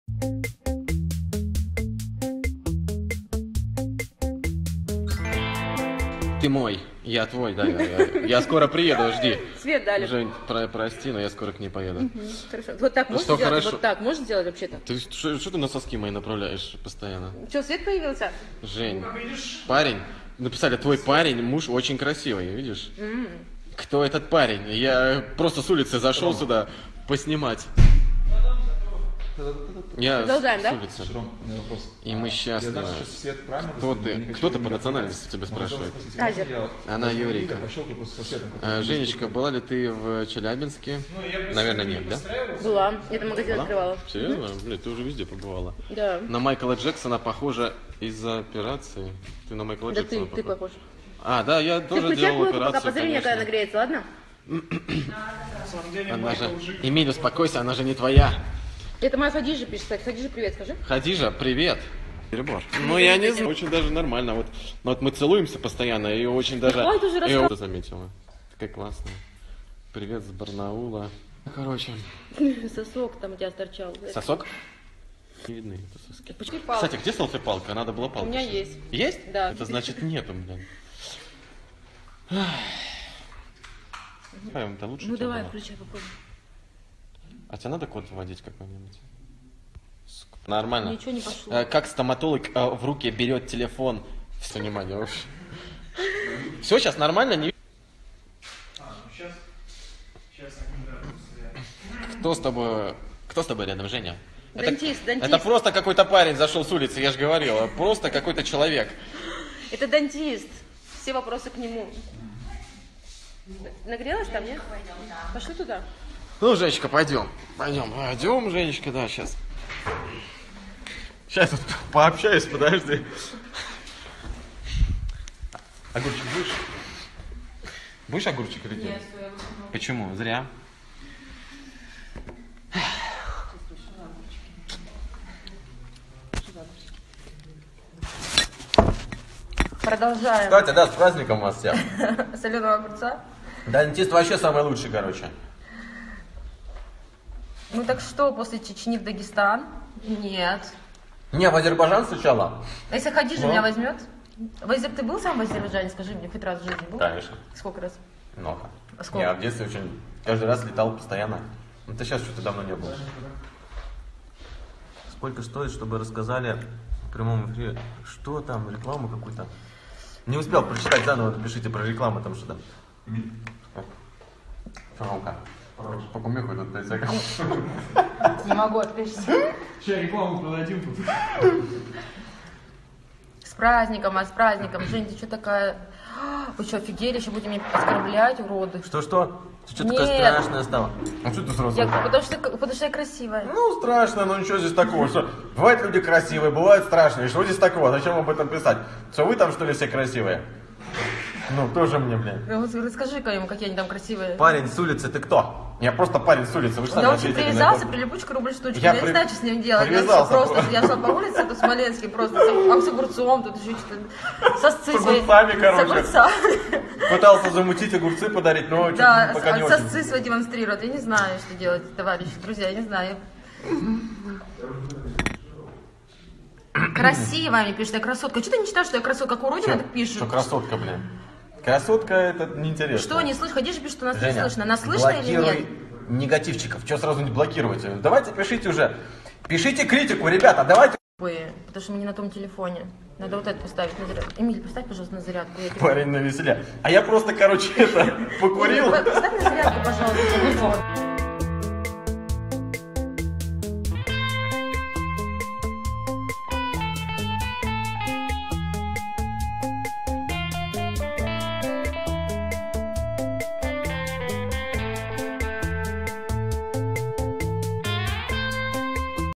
ФЕОФИЛАКТОВА И КРАСАВЧИК ЭМИЛЬ НА ДЕНЬ ВСЕХ ВЛЮБЛЕННЫХ (ondom2.com)
В этом выпуске новости и слухи  про Евгению Феофилактову.